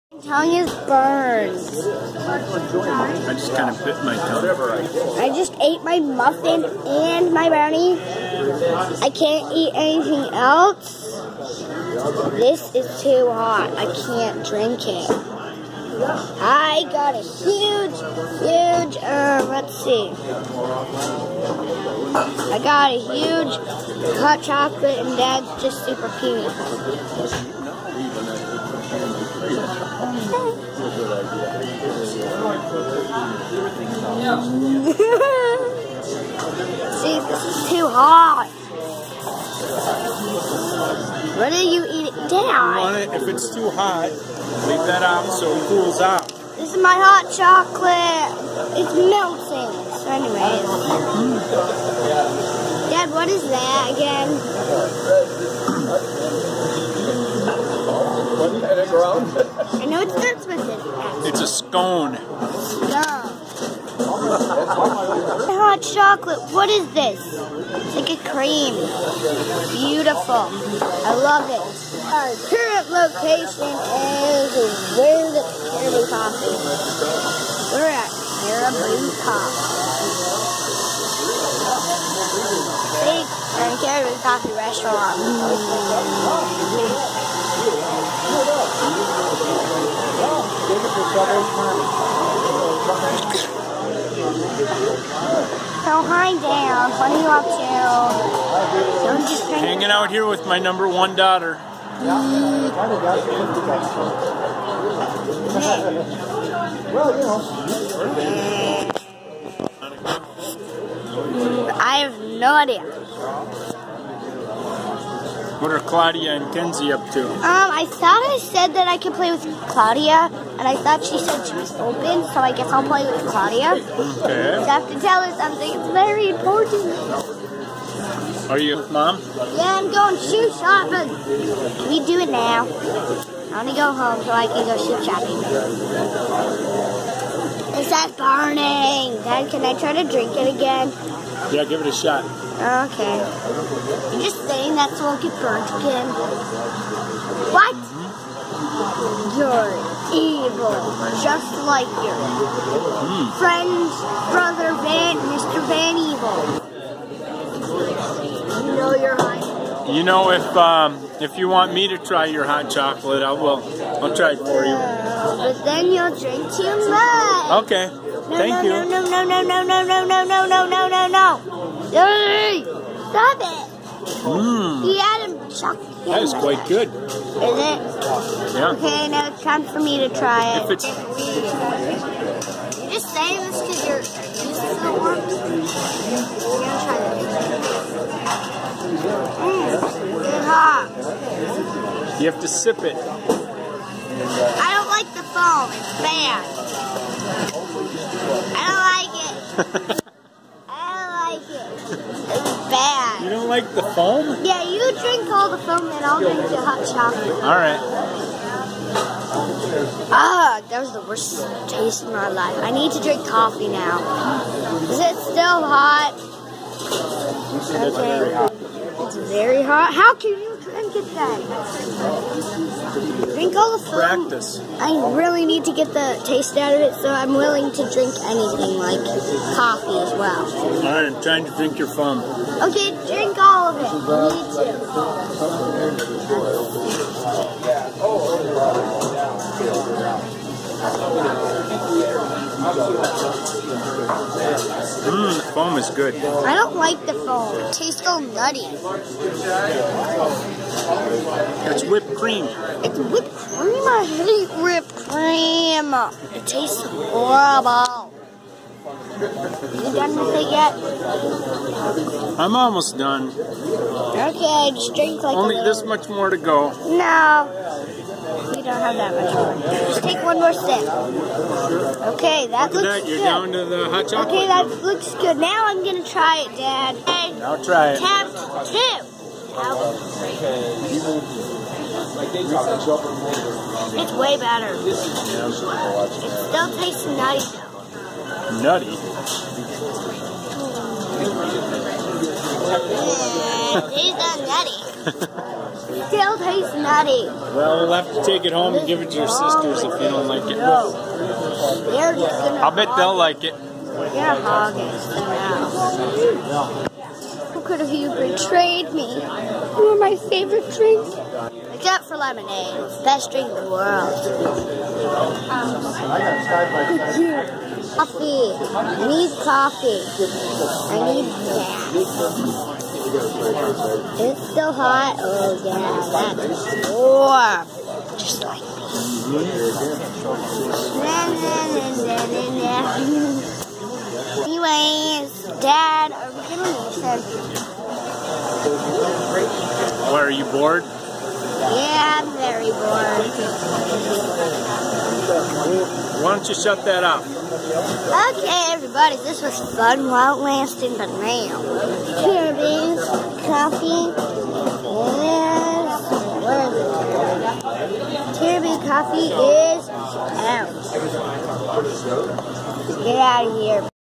Caribou Coffee The story of the hot 🍫
Me in Caribou Coffee I hate my hot 🍫